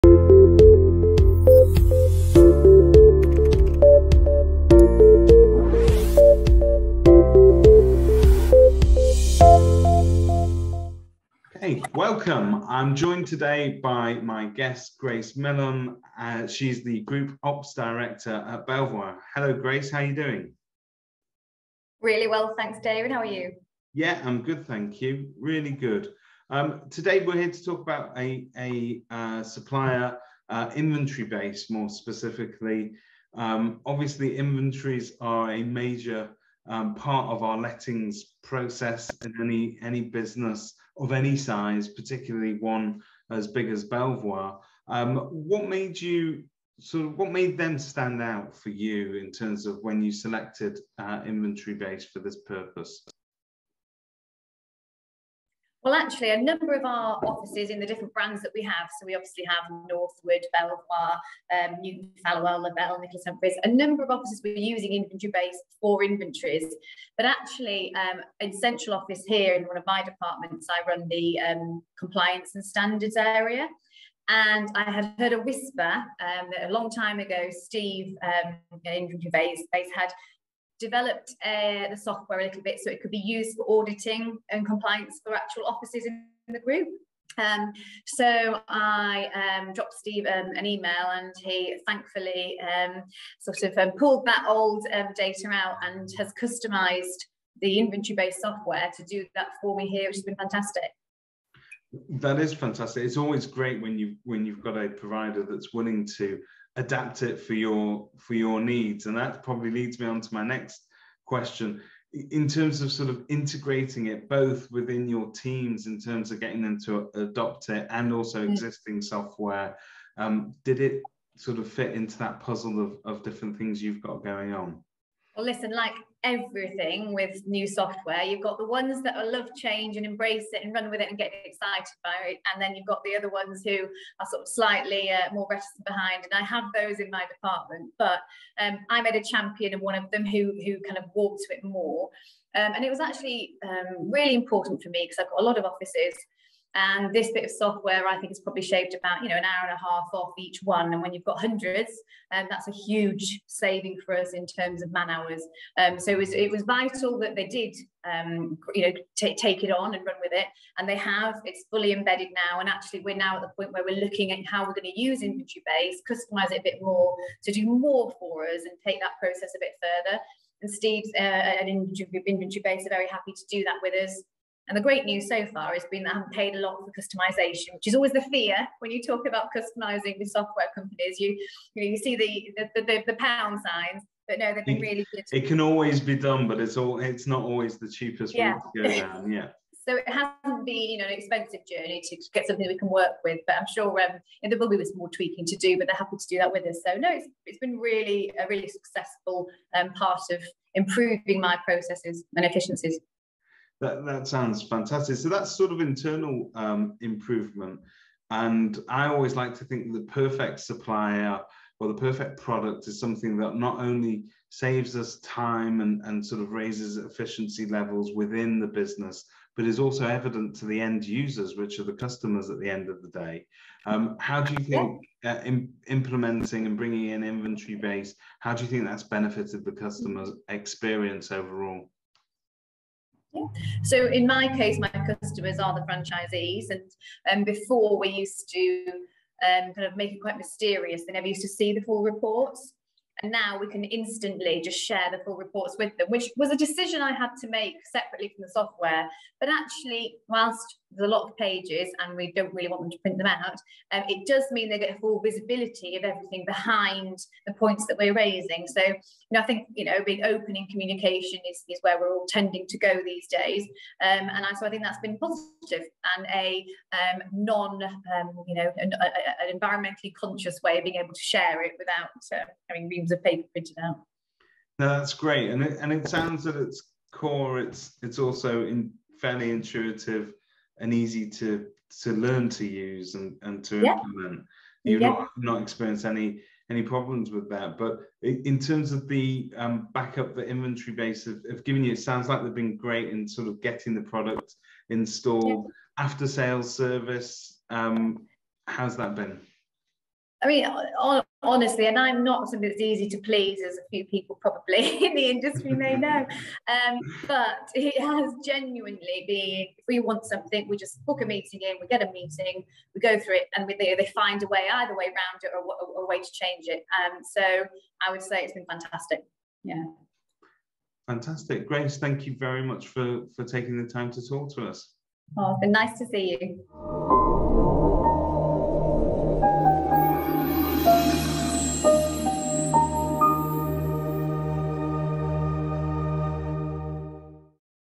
Kerfuffle Agent Testimonial - Inventory Base
David talks to Grace Milham, from The Belvoir Group, about how Inventory Base has helped her business.

Could they help yours too?